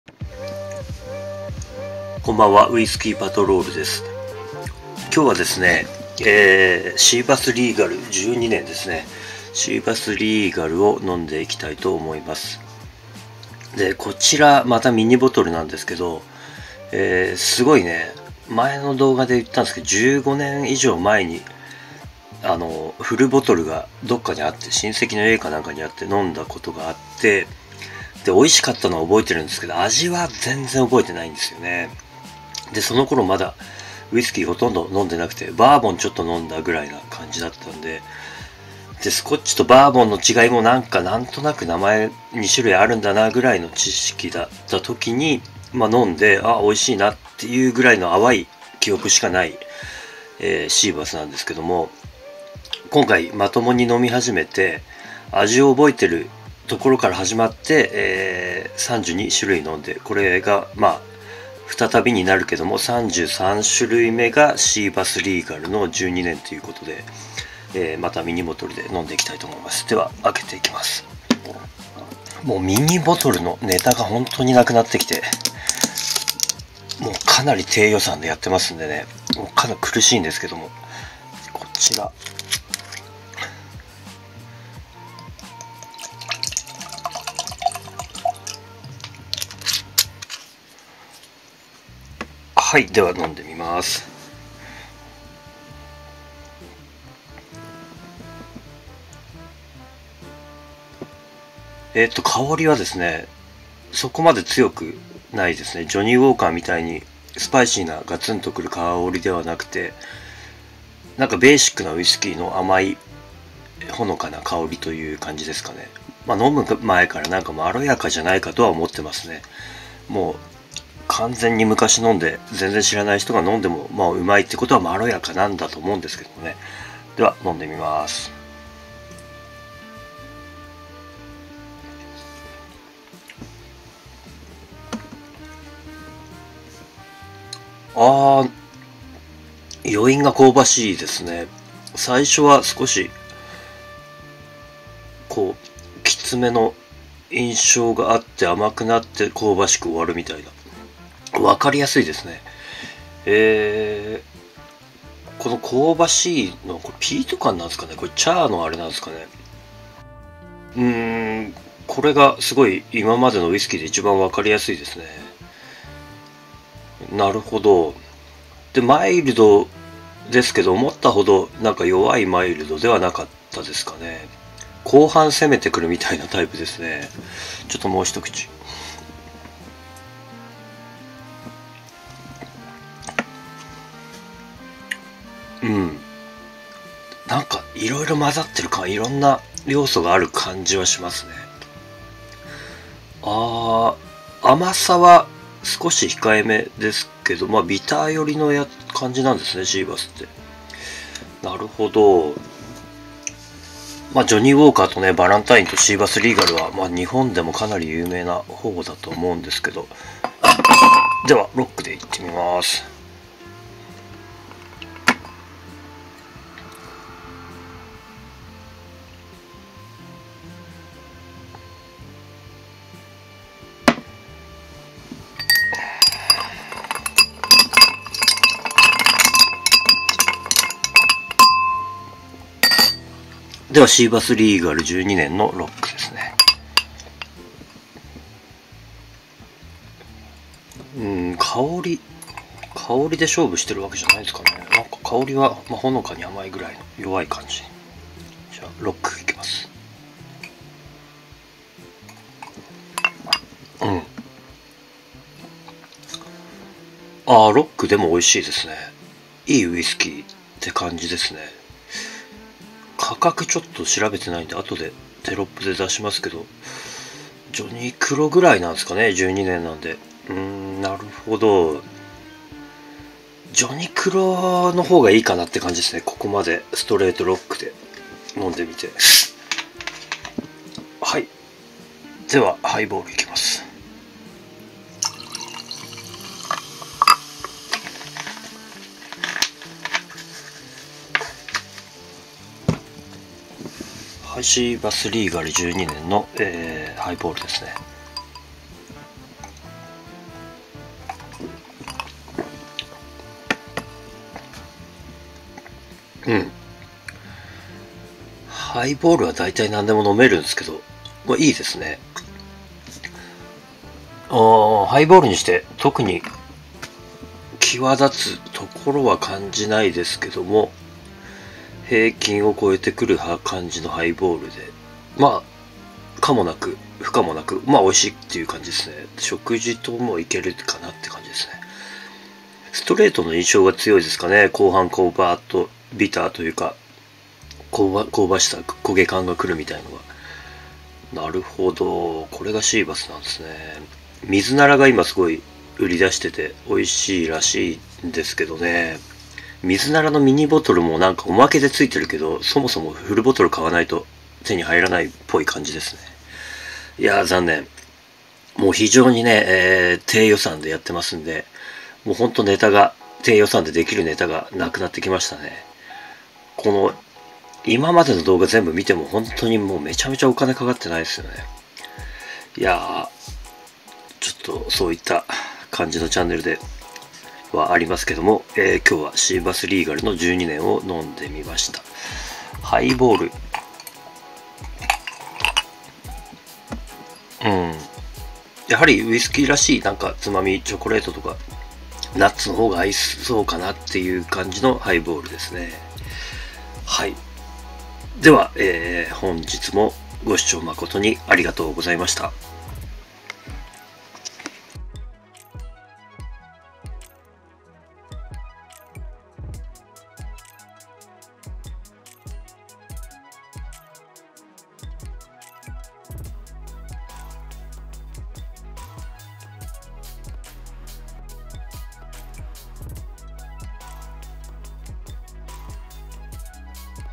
シーバスリーガル飲んでみた🥃スコッチの象徴12年
素人によるウイスキーのレビューです🥃

Amazonで購入→

もしウイスキーをリストから送っていただけたら動画にさせていただきます。
ほしいものリスト🥃　wish list🥃

山崎25年　Amazonで購入する

山崎12年　Amazon